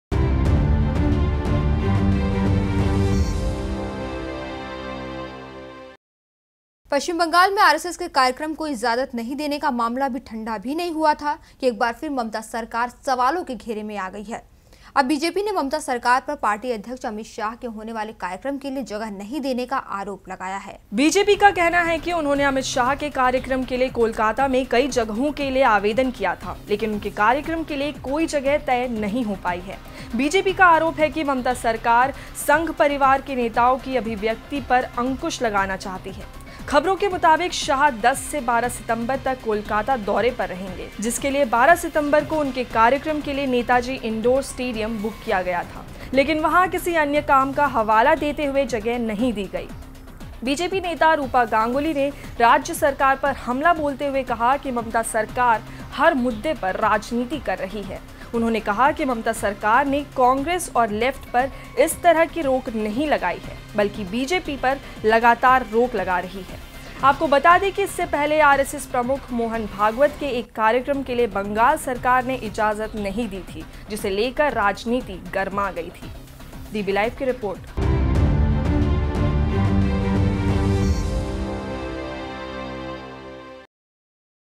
ममता के राज में अमित शाह की नों एंट्री । Mamta cancelled the bookings of amit shah.
पश्चिम बंगाल में आरएसएस के कार्यक्रम को इजाजत नहीं देने का मामला अभी ठंडा भी नहीं हुआ था कि...एक बार फिर ममता सरकार सवालों में घेरे में घिर गई हैं। अब बीजेपी ने ममता सरकार पर पार्टी अध्यक्ष अमित शाह के होने वाले कार्यक्रम के लिए जगह नहीं देने का आरोप लगाया है।

बीजेपी का कहना है कि उन्होंने अमित शाह के कार्यक्रम के लिए कोलकाता में कई जगहों के लिए आवेदन किया था लेकिन उनके कार्यक्रम के लिए कोई जगह तय नहीं हो पाई है। बीजेपी का आरोप है कि ममता सरकार, संघ परिवार के नेताओं की अभिव्यक्ति पर अंकुश लगाना चाहती है। खबरों के मुताबिक शाह 10 से 12 सितंबर तक कोलकाता दौरे पर रहेंगे। जिसके लिए 12 सितंबर को उनके कार्यक्रम के लिए नेताजी इनडोर स्टेडियम बुक किया गया था,  लेकिन वहां किसी अन्य काम का हवाला देते हुए जगह नहीं दी गई। बीजेपी नेता रूपा गांगुली ने राज्य सरकार पर हमला बोलते हुए कहा कि ममता सरकार हर मुद्दे पर राजनीति कर रही है। उन्होंने कहा कि ममता सरकार ने कांग्रेस और लेफ्ट पर इस तरह की रोक नहीं लगाई है बल्कि बीजेपी पर लगातार रोक लगा रही है। आपको बता दें कि इससे पहले आरएसएस प्रमुख मोहन भागवत के एक कार्यक्रम के लिए बंगाल सरकार ने इजाजत नहीं दी थी। जिसे लेकर राजनीति गरमा गई थी।